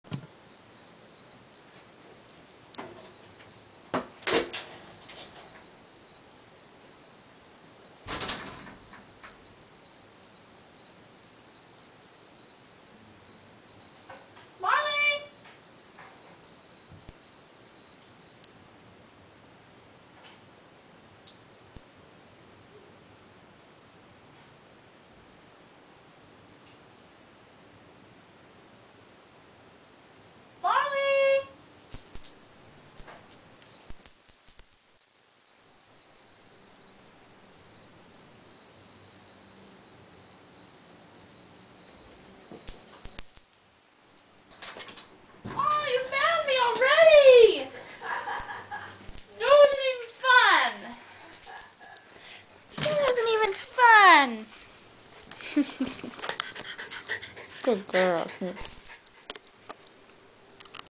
Jack Russell hide and seek
Playing hide and seek with my JRT. She remembers where I always hide